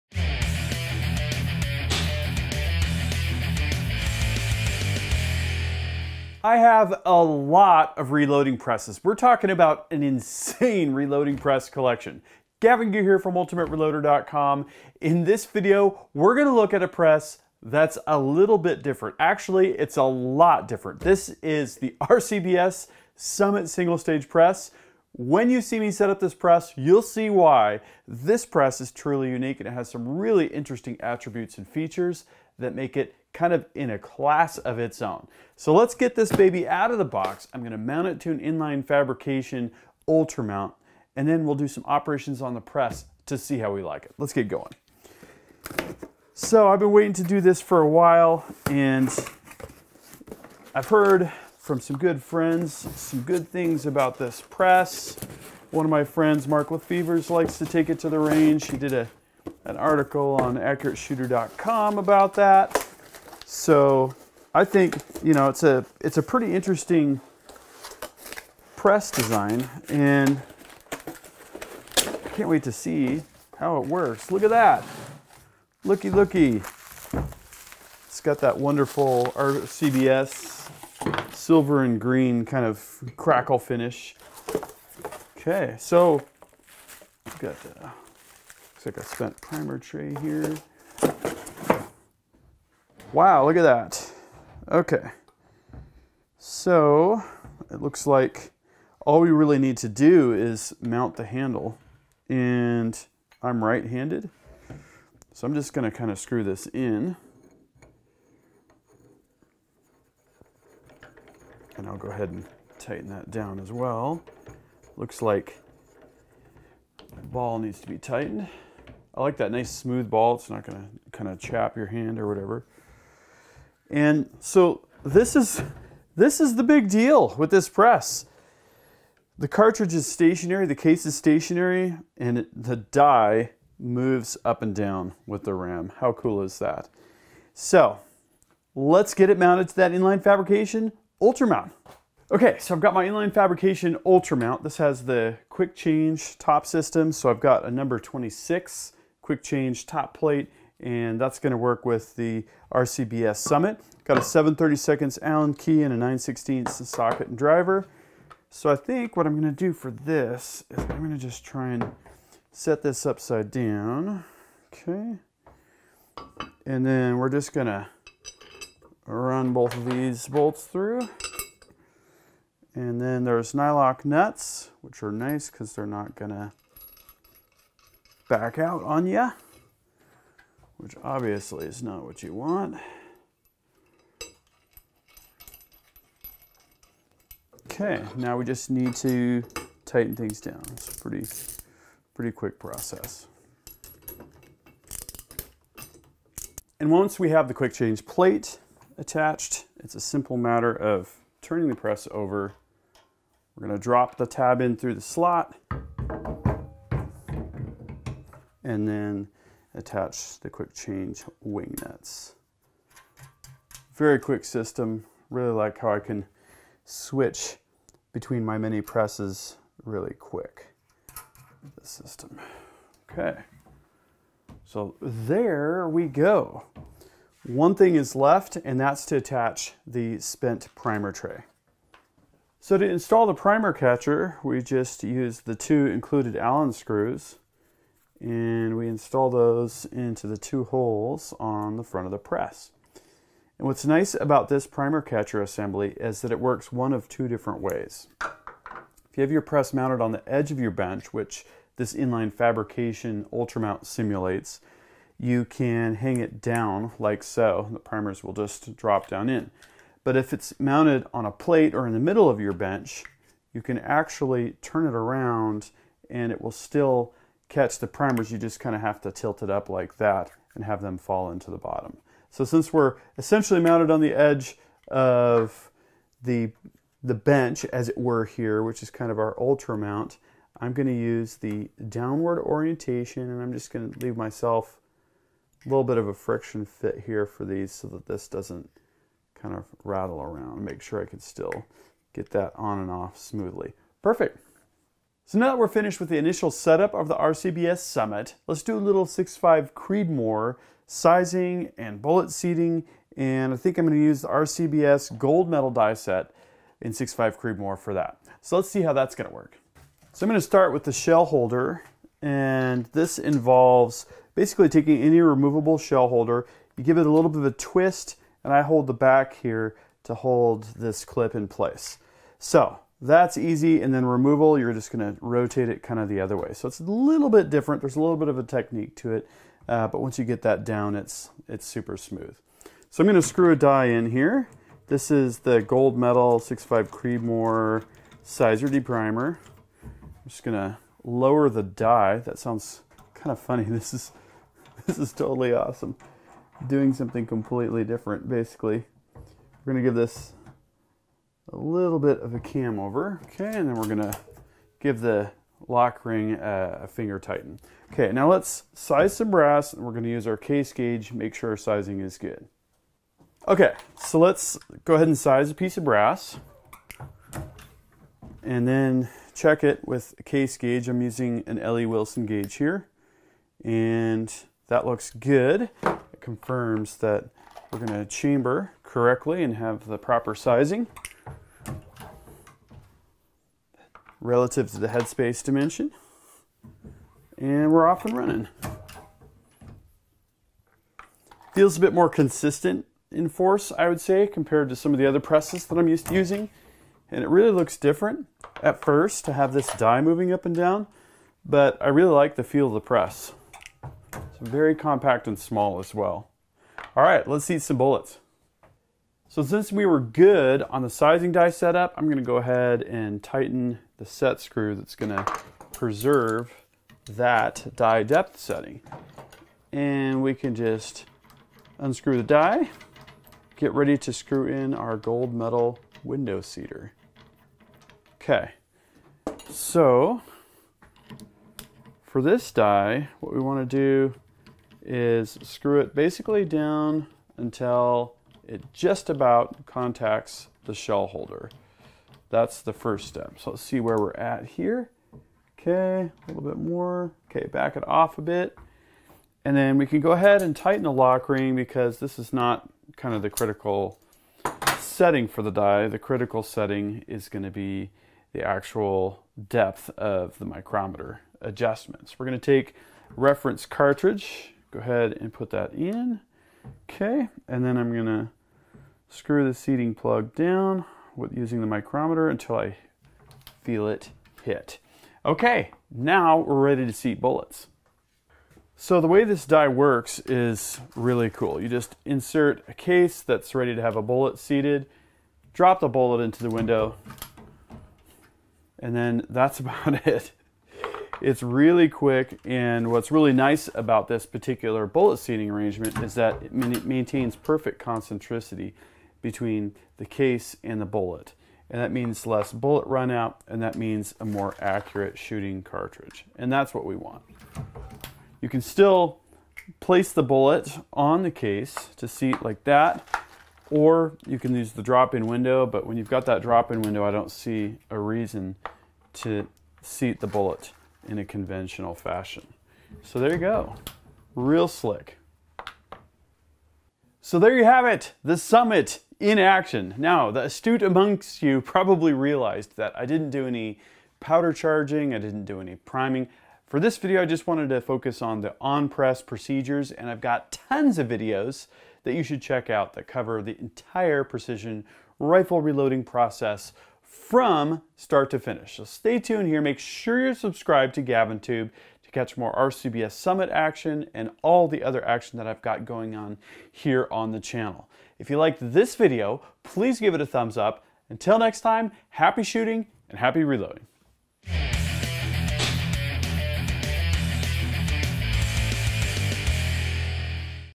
RCBS Summit Press: Unboxing and Overview, Loading 6.5 Creedmoor
The RCBS Summit is far from conventional! In this video, I unbox the RCBS Summit, set it up, and go through some of the processes involved in loading 6.5 Creedmoor.

Ultimate Reloader LLC / gavintoobe Material Connection Notice:

The following Ultimate Reloader partners are featured in this video:
- RCBS